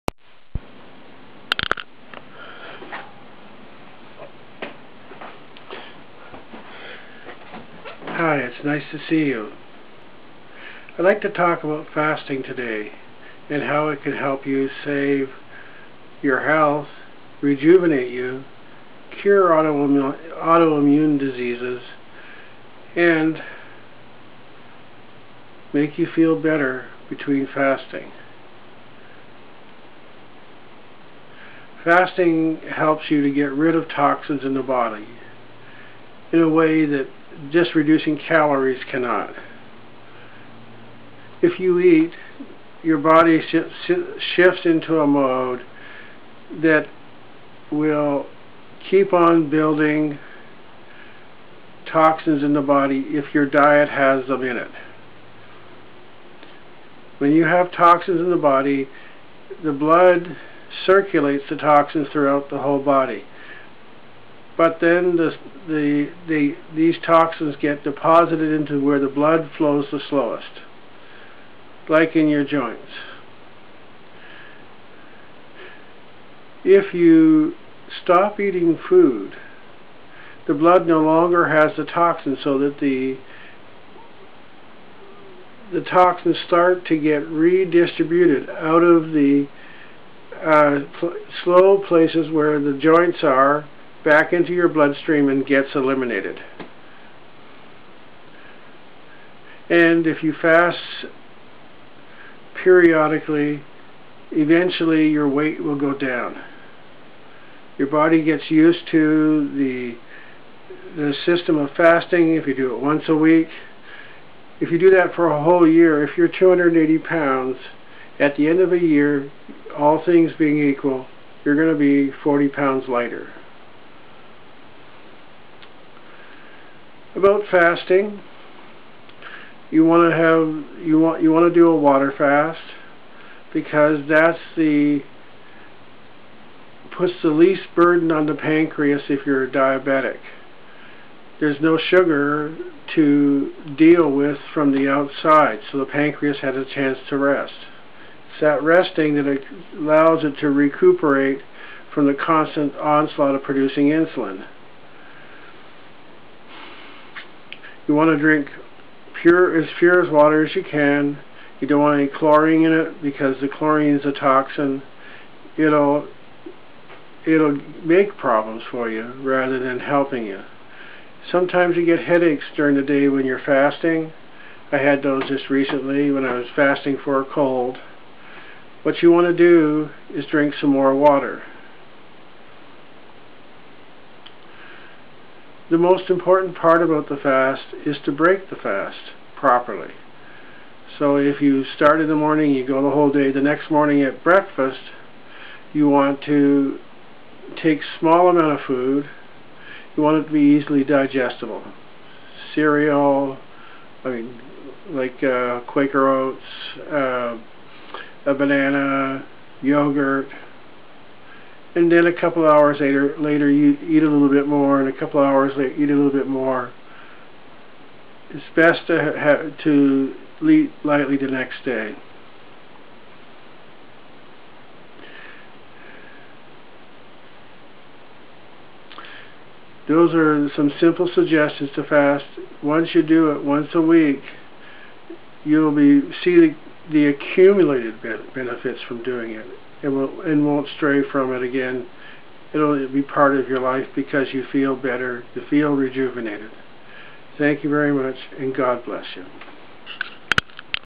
fountain of youth and cancer free
Learn how fasting can make you younger. When you fast on water only, your body switches from processing sugar to processing fats and proteins from the body. This takes about 18 hours. When the body is processing protein from from your cells, it cannibalizes the weakest cells. That means that  of all the cells in your body, the weakest and poorest malfunctioning cells, including cancer cells that will not show up on scans, become food for the body.

After the fast, the percentage of damaged DNA cells to correct DNA copies of cells goes higher.

Skin cells that produce small amounts of collagen, are consumed in this fasting, leaving you with good skin cells,. In this way  your skin becomes younger and smoother.

It is this repeated